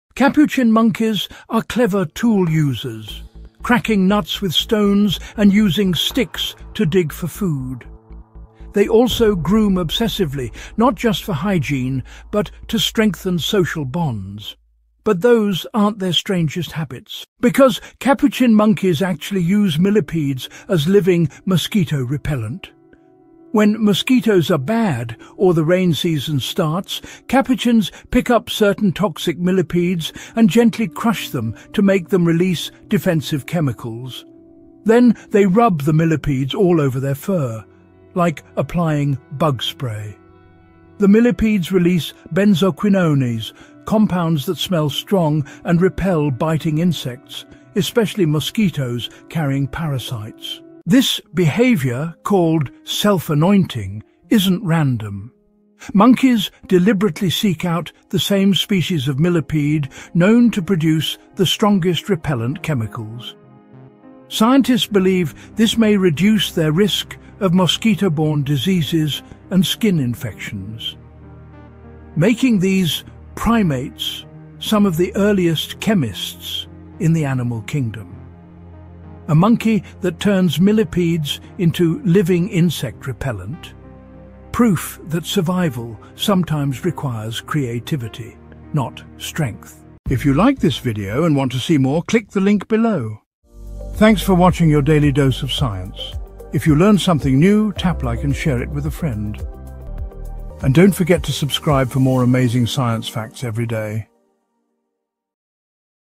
The Capuchin Monkey uses millipedes as Mosquito Repellent?! Wild!
Capuchin monkeys have some of the strangest survival tricks in the animal kingdom — including using millipedes as living mosquito repellent. When bug season hits, they pick up toxic millipedes, crush them lightly, and rub the chemicals all over their fur like natural insect spray. These compounds, called benzoquinones, help protect them from mosquitoes, parasites, and skin infections. Nature always finds a clever way — and these monkeys might just be the world’s first wild chemists.